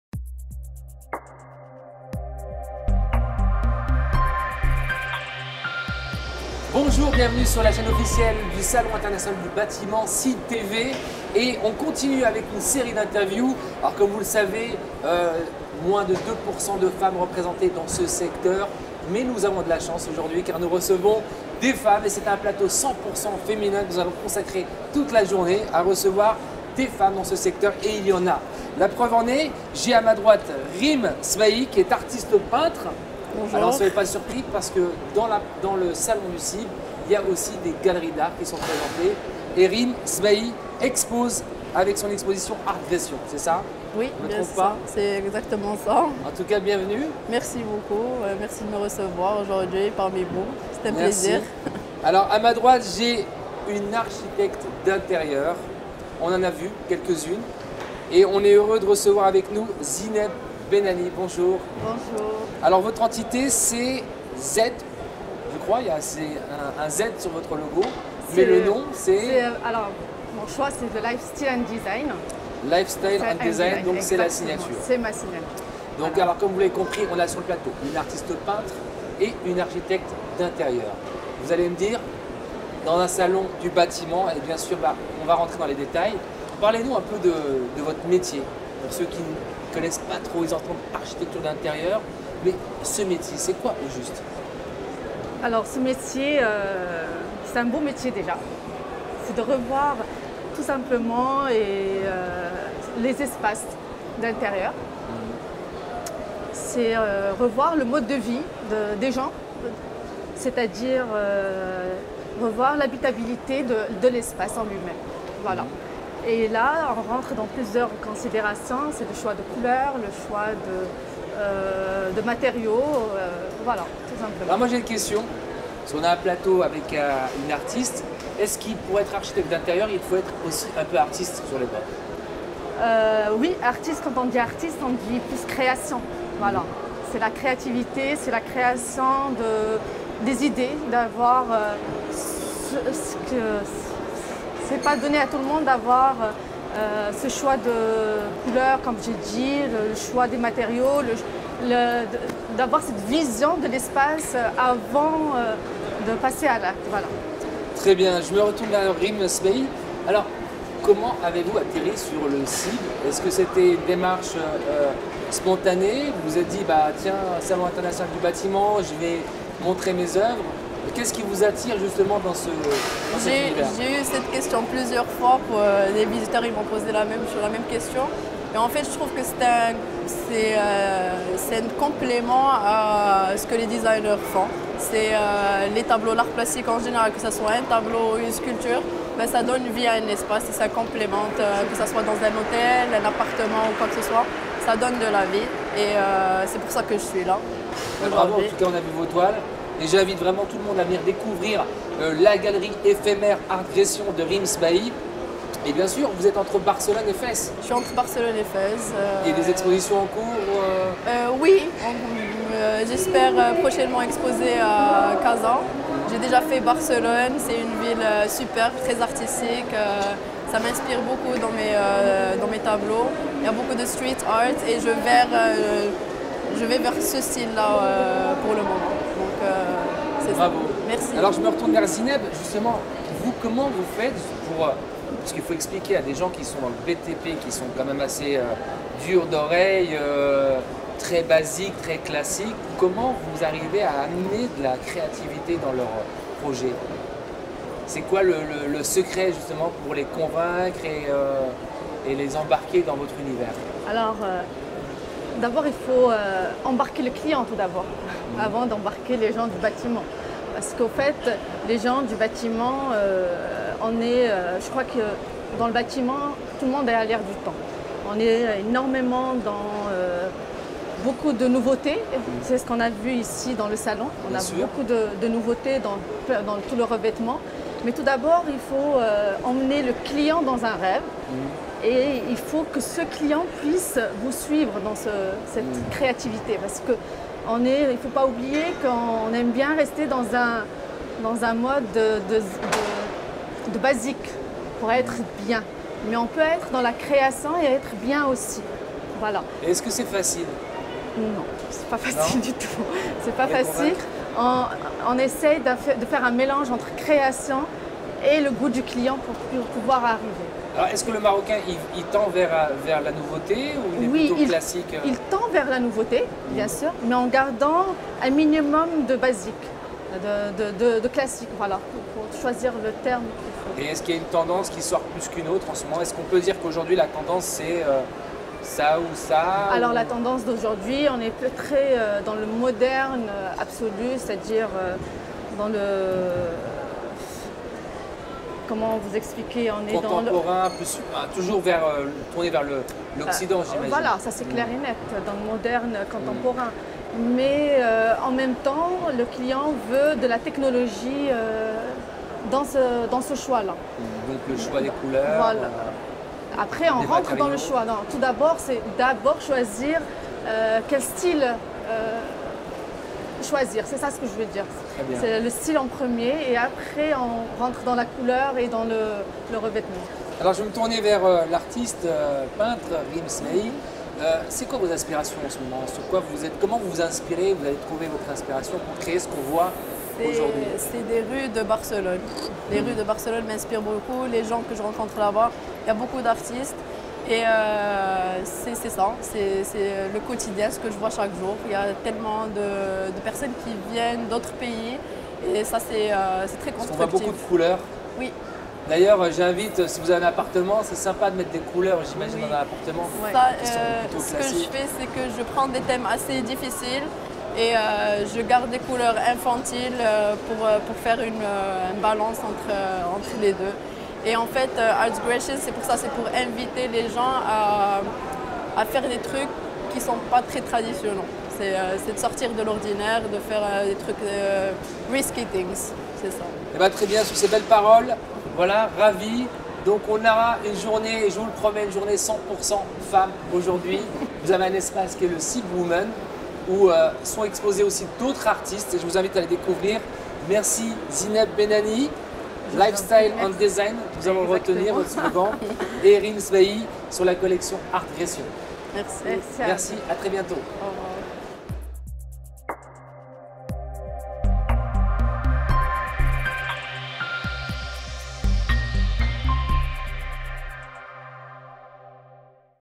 SIB TV Interview Rime SBAI, Artiste Peintre et Zineb Bennani, architecte d'intérieur
Rencontre entre deux passionnés d'art et d'architecture d'intérieur.
Le  Salon International du Bâtiment 2018 a accueilli l' exposition des oeuvres de  l'artiste Rime Sbai